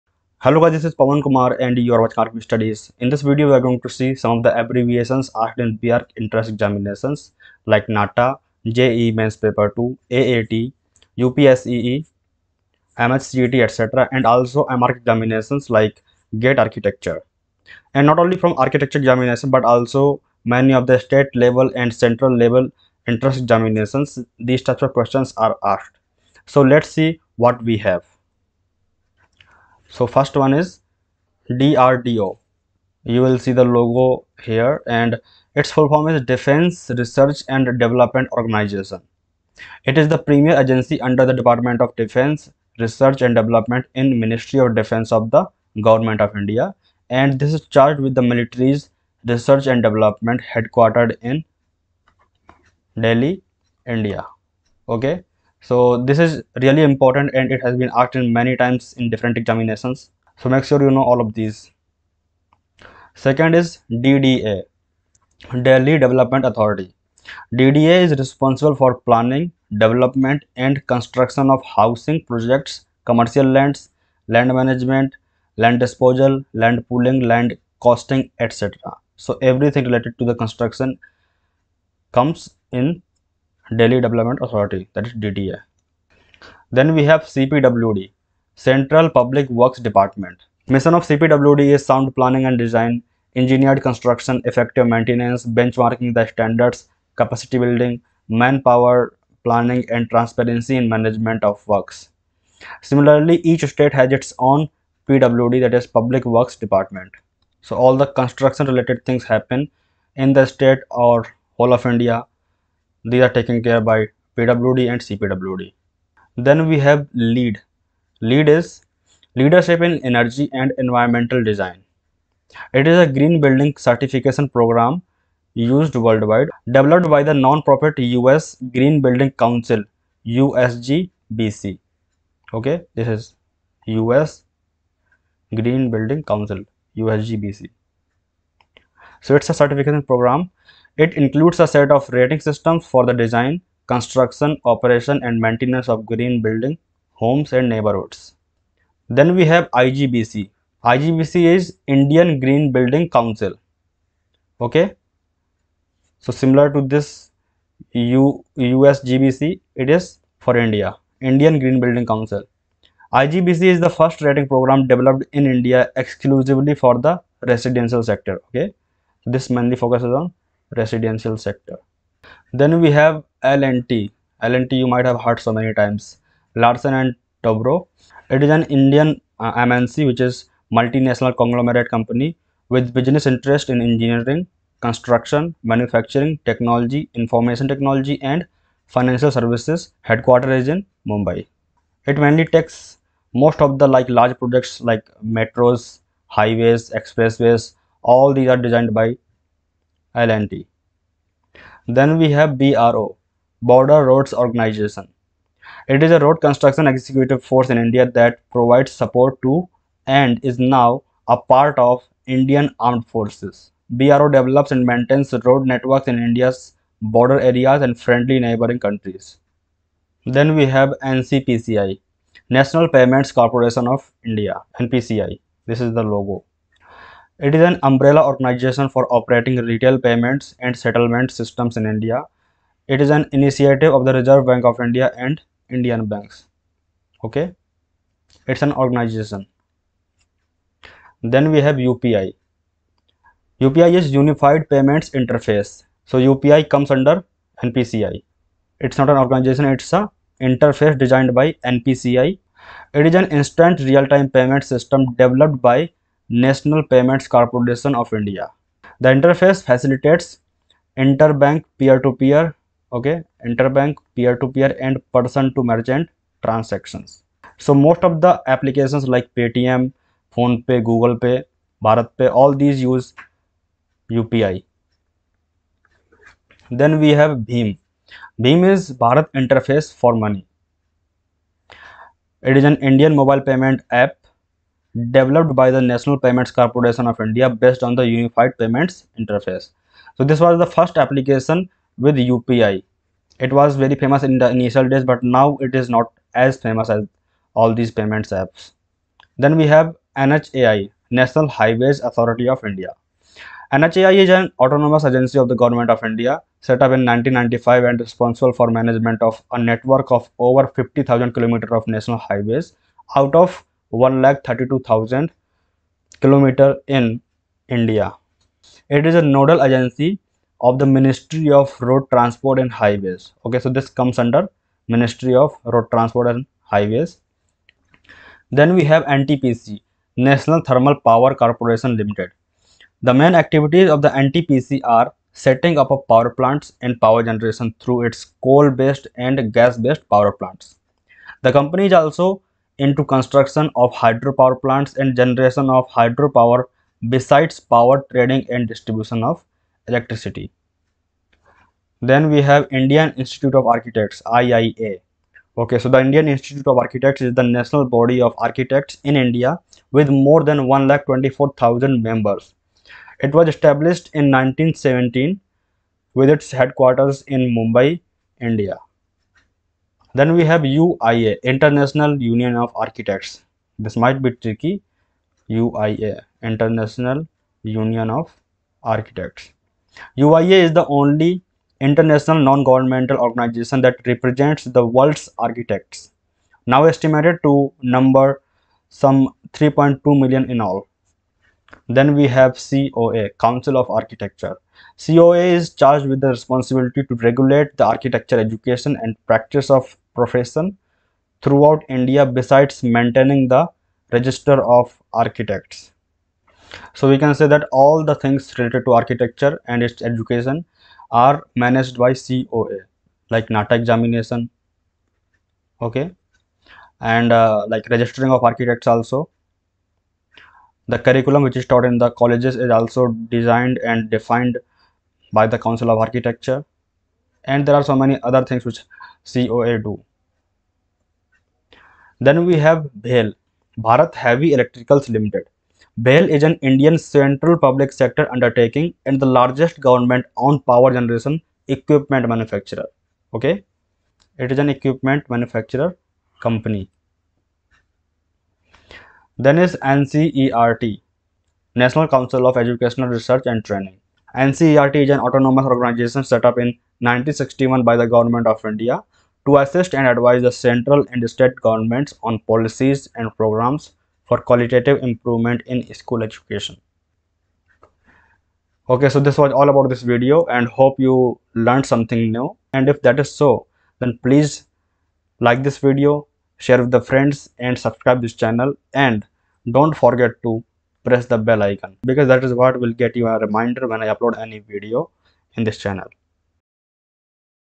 Abbreviations (Full forms) asked in B.Arch. Exams | NATA | JEE Mains Paper 2 | GATE Aptitude Section
This video covers some of the most frequently asked abbreviations in B.Arch. Examinations like NATA, JEE Mains Paper 2, Gate Architecture, AAT etc. Some of these have already been asked in these examinations so make sure you know these as these can be asked in aptitude section of NATA, JEE Mains Paper 2.